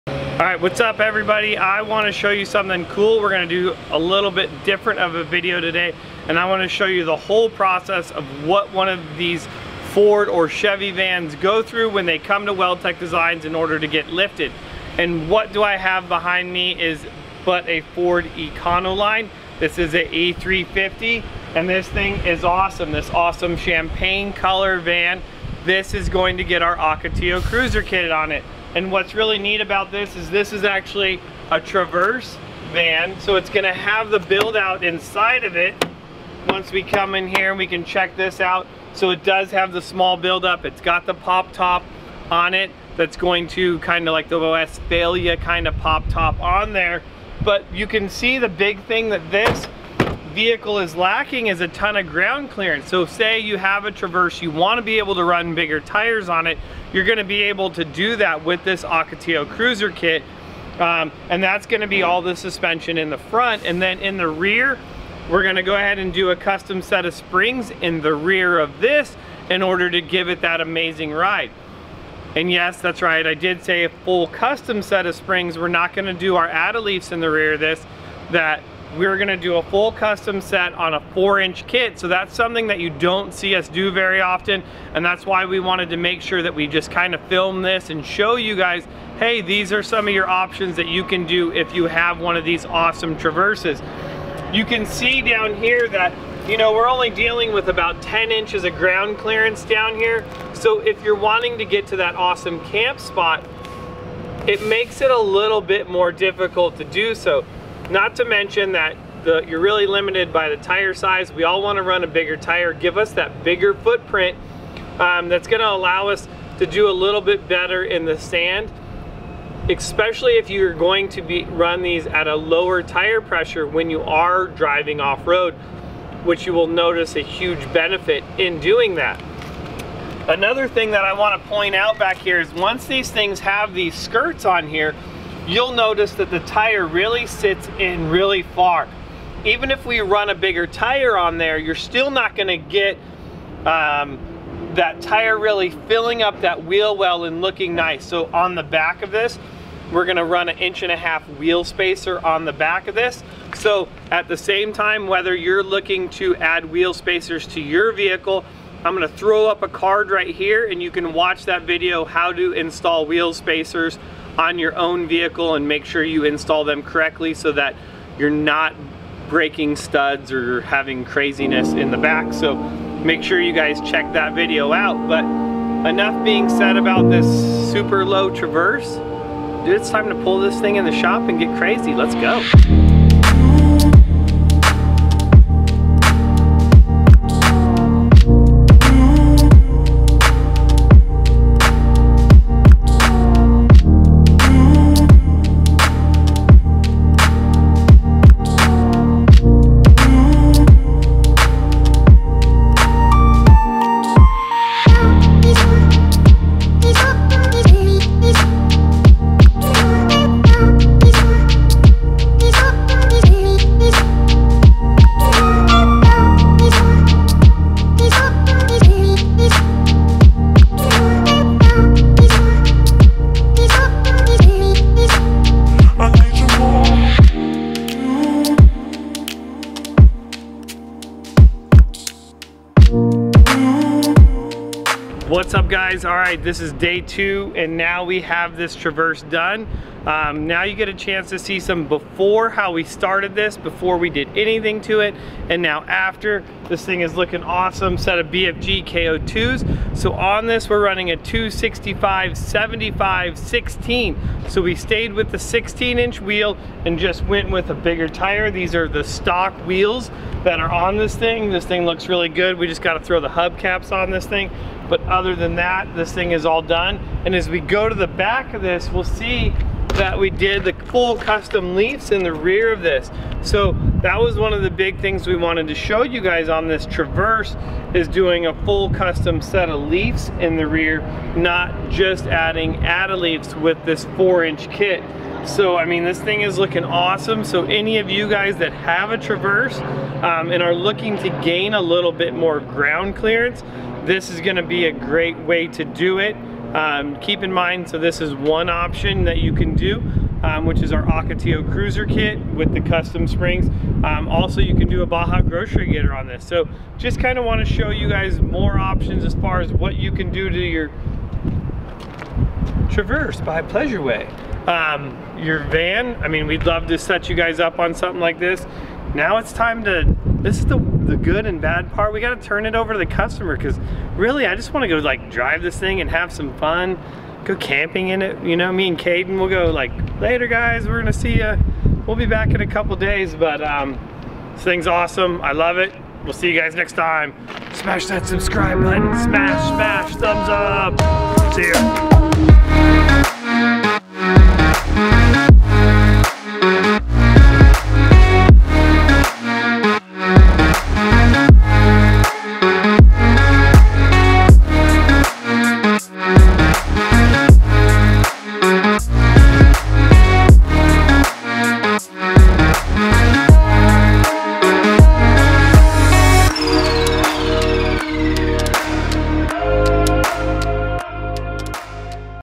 Ford Econoline Camper Van Gets 4" Lift
In this video we take a look at this Ford Econoline Camper van we recently lifted with our 4" Lift. This camper van also got custom rear leaf springs for the best possible ride out of the rear rather than going to lift blocks which will not give you the ride custom rated springs will! This lift is perfect for those who aren't looking to go a full 6" of lift but want to run a little bigger of a tire. On this Ford Econoline we went with a 265/75 16 BFG Ko2 tire. This rig is sitting and looking awesome after the lift and wheels. This Ford Traverse camper van also has an awesome build out in the inside perfect for a couple of 2 or a solo van lifer.

For more info on the products we use to make these awesome vans or social content check out: